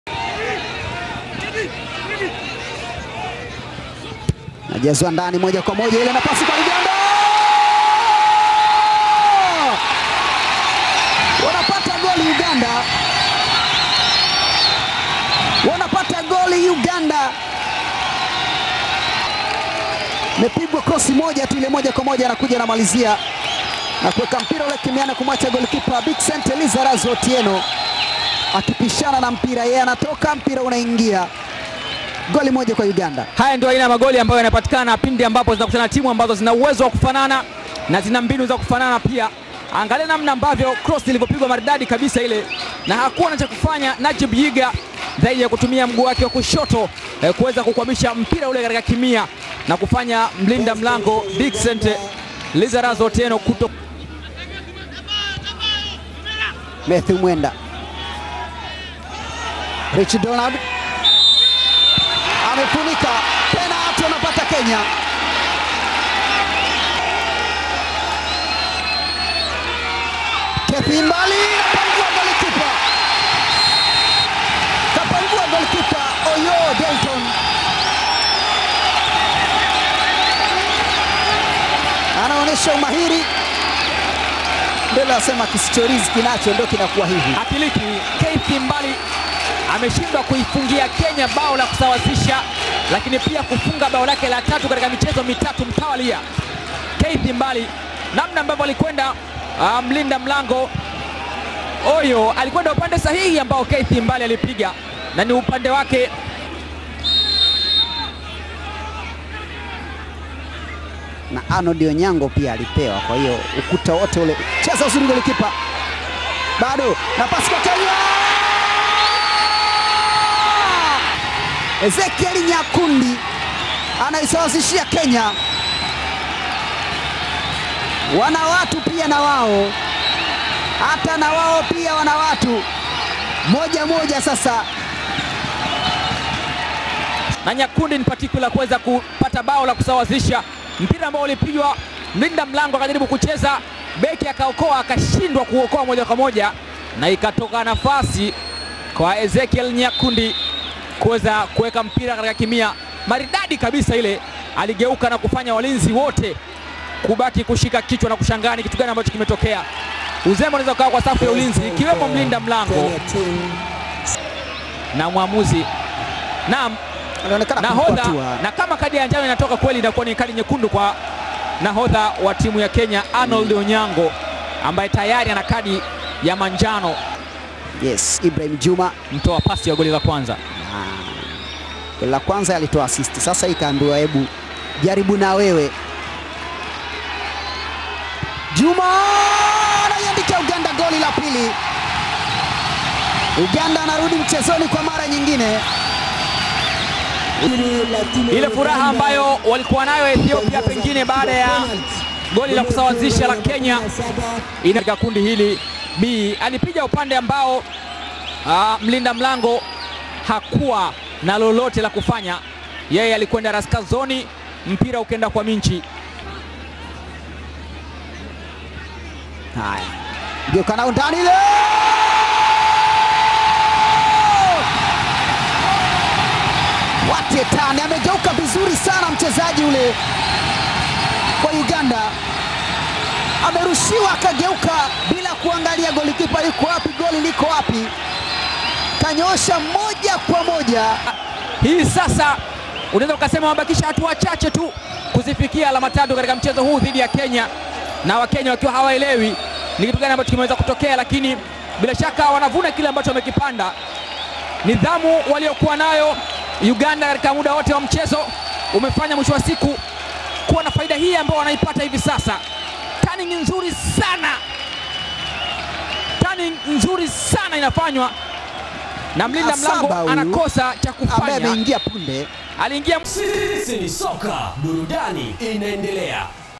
MAGOLI YOTE: KENYA 1-3 UGANDA (CECAFA U17 AFCON QUALIFIER)
Ni katika michuano ya kuwania kufuzu AFCON kwa vijana wenye umri chini ya miaka 17, kutoka ukanda wa CECAFA, inayoendelea Jijini Dar es salaam.

Katika mchezo huu uliopigwa  kwenye Dimba la Chamazi Complex, mabao ya Uganda yamefungwa na iga Najib 45', Ibrahim Juma 79', Asaba Ivan 83' huku la Kenya likifungwa na Ezekiel Nyakundi 74'.